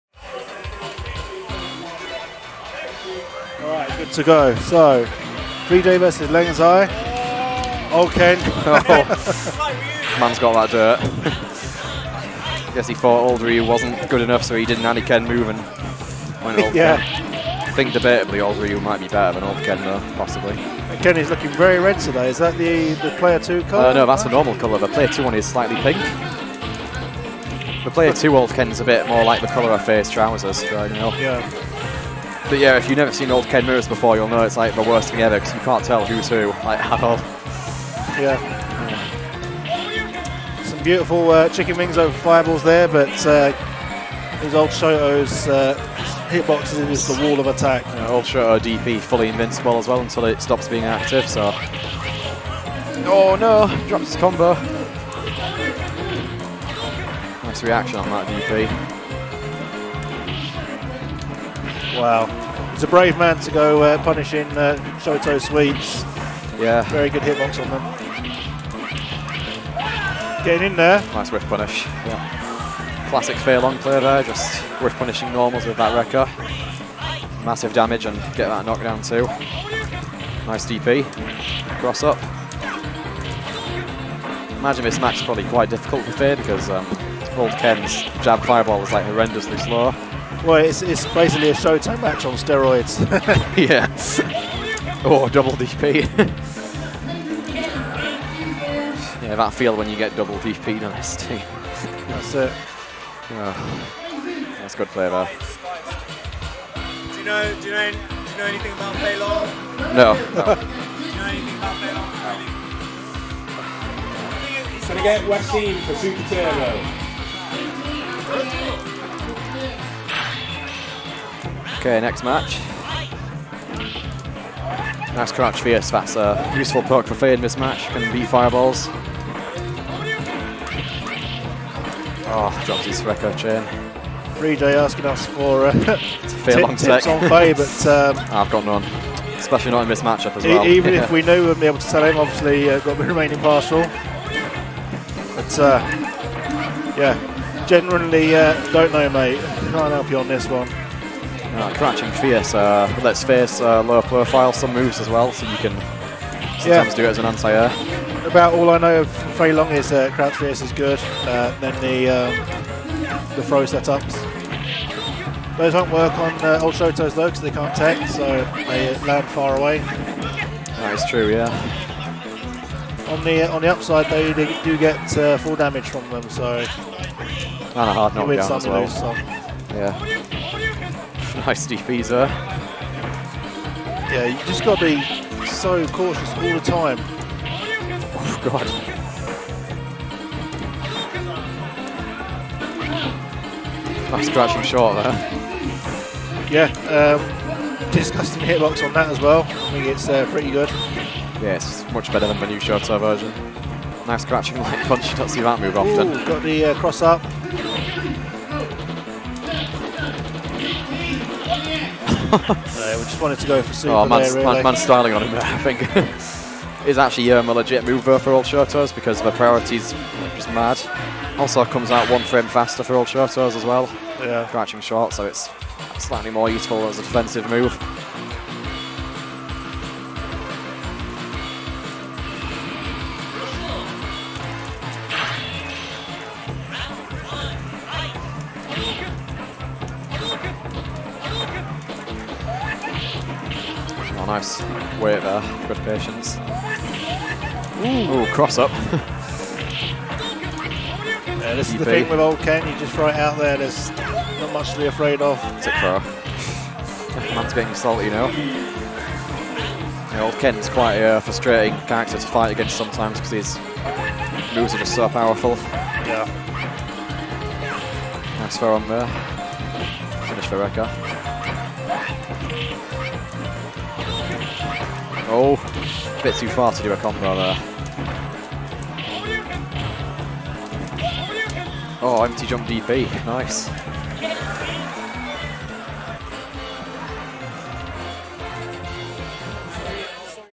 Fist of the Notts Star 2 - Losers Round 2 - 3J VS Leng Zai
Held at Nottingham Ice Arena, hosted by Robin Hoods and Dudley Boyz.  ST tournament run by Grand Master Challenge.